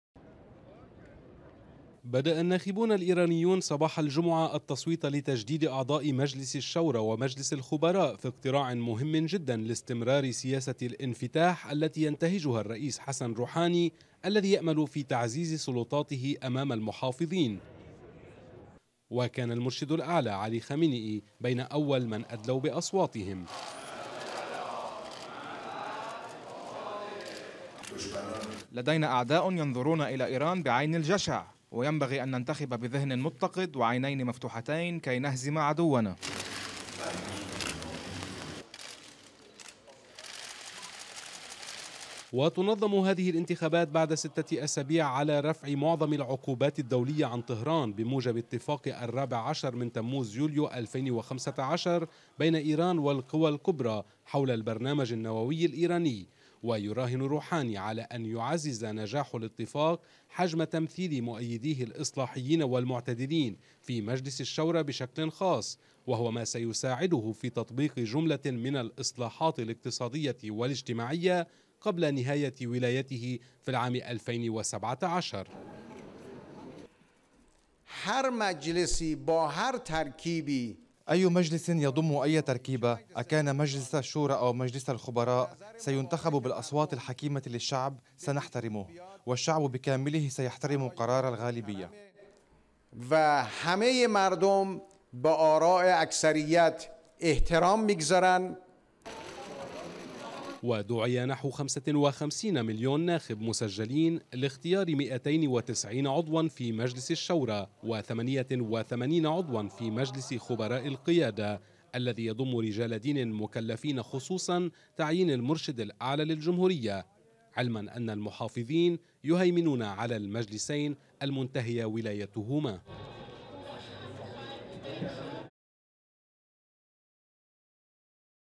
الايرانيون يصوتون لانتخاب اعضاء مجلسي الشورى والخبراء
بدأ الناخبون الايرانيون صباح الجمعة التصويت لتجديد اعضاء مجلس الشورى ومجلس الخبراء في اقتراع مهم جدا لاستمرار سياسة الانفتاح التي ينتهجها الرئيس حسن روحاني الذي يأمل في تعزيز سلطاته امام المحافظين.نقرير مسجل: باهار شوغي، شربل باخوس.الرجاء قراءة التعليمات والتقيد بها.